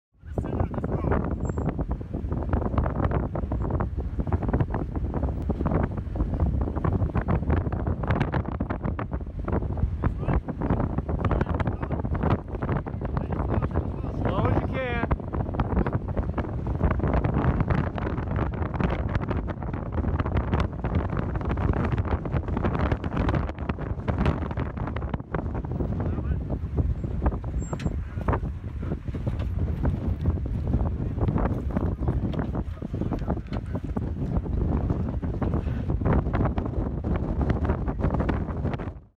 Raw footage: David's Military Tribute Jeep
David's Military Tribute Jeep coming down "Horror Hill" on the Jackrabbit trail near Victorville.
Only simple color balance & correction and basic stabilization added